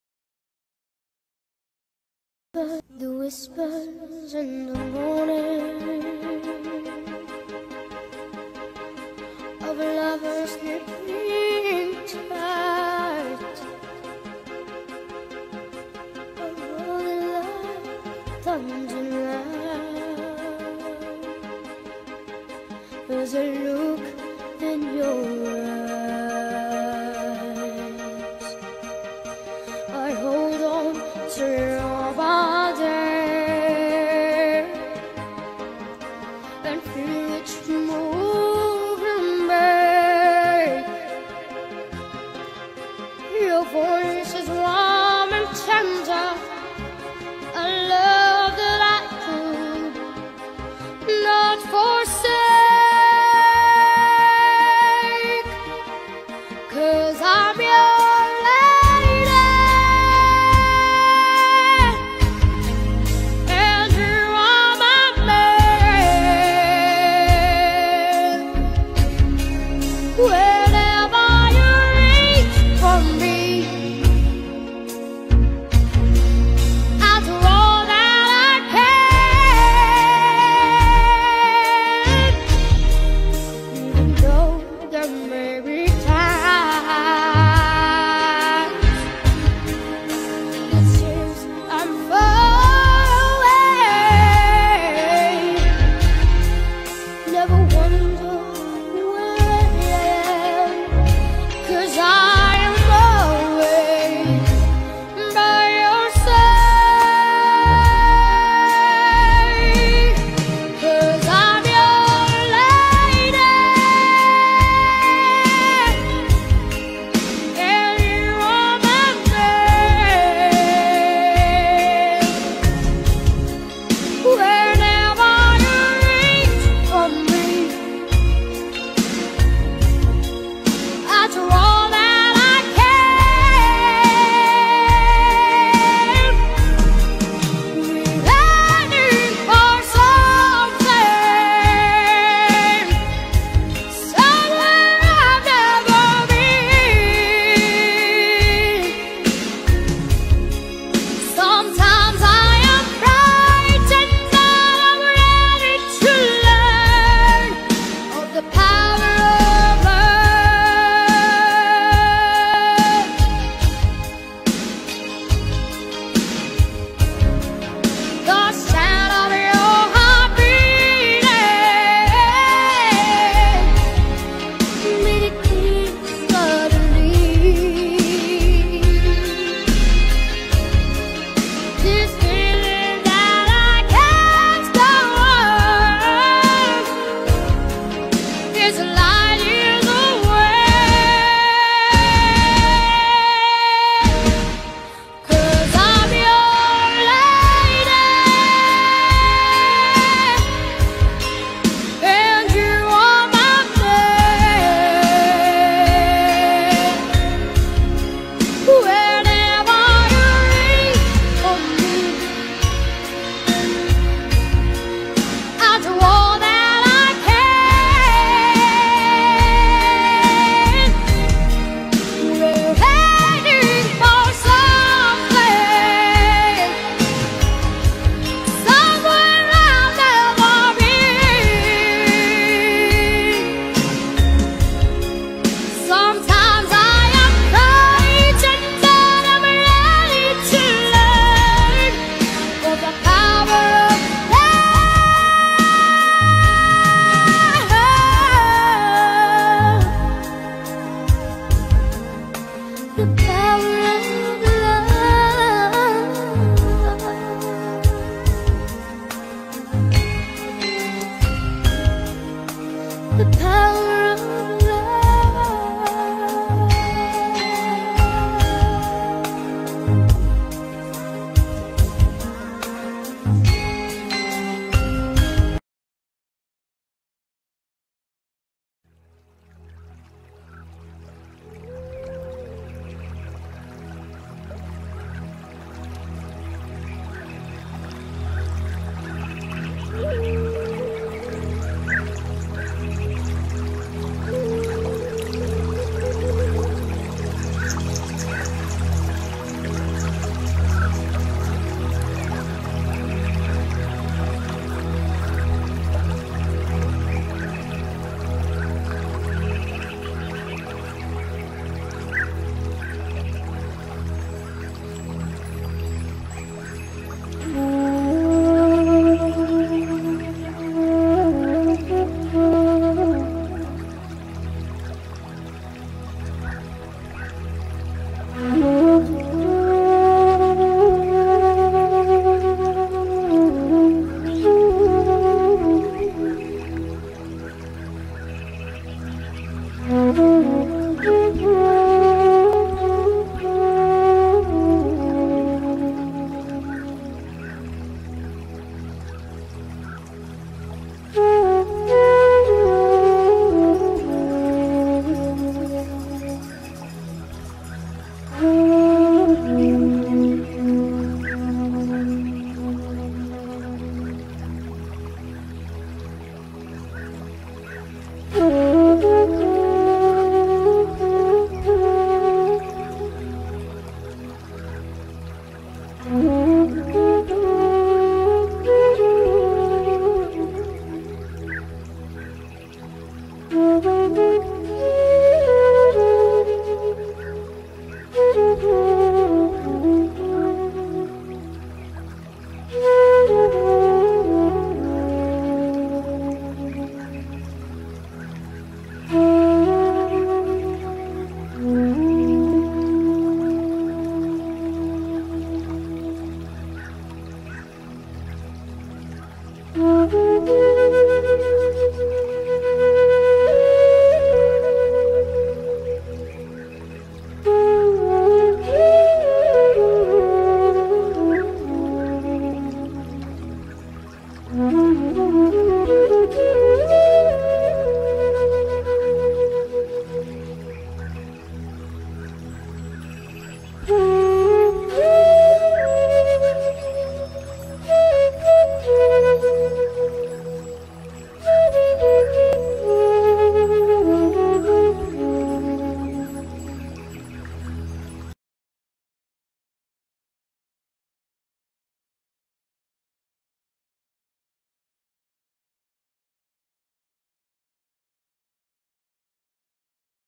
Humour ; Rétrospective des actualités de l'année 2016. HD
Photomontages sur des événements marquants de l’année 2016. Une autre façon de voir l’actualité. A voir en « PLEIN ECRAN ». Blog ;